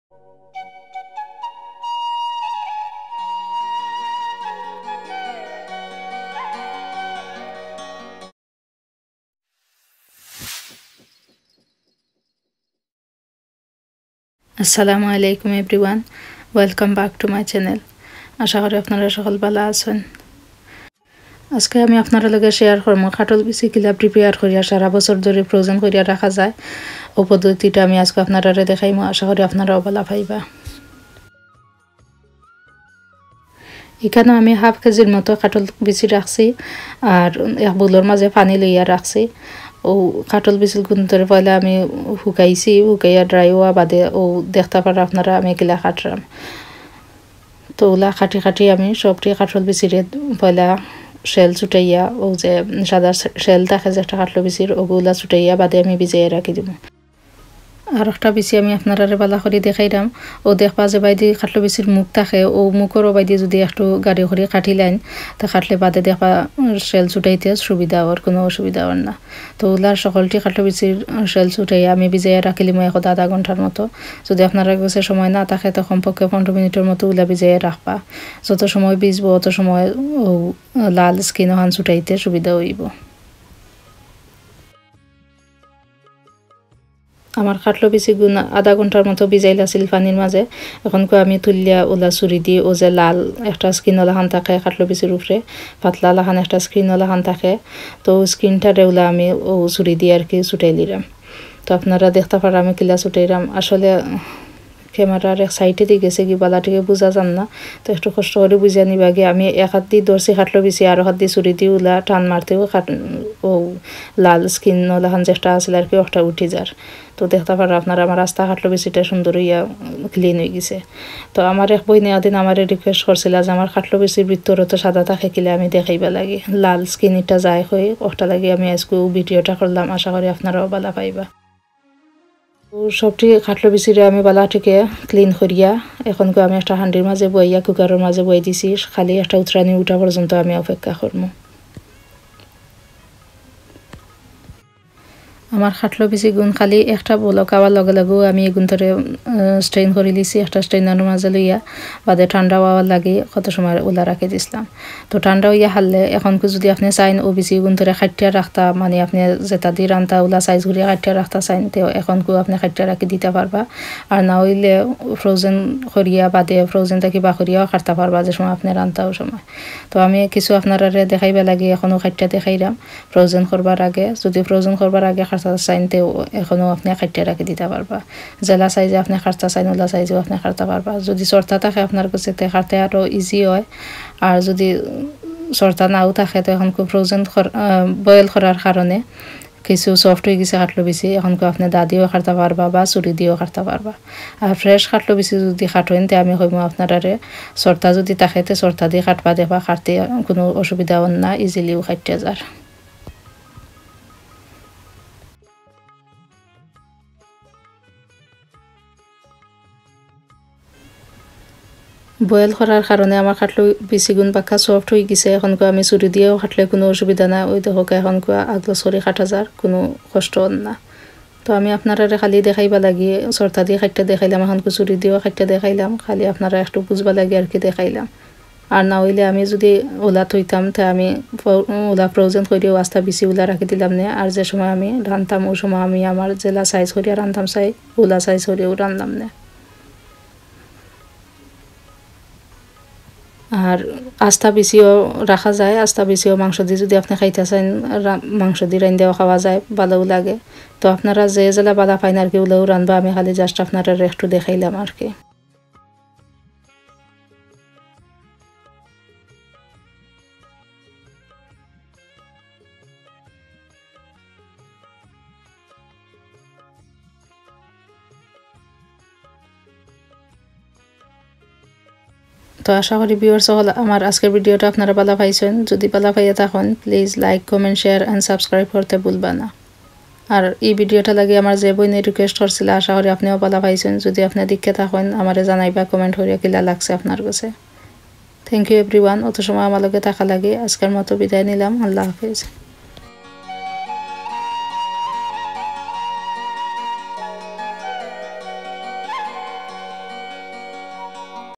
Katal Bisi: Preparation | PSR
Hello viewers! Welcome back to another PSR Video on the YouTube Channel! I hope you are all well.

In today’s video we are preparing Katal Bisi.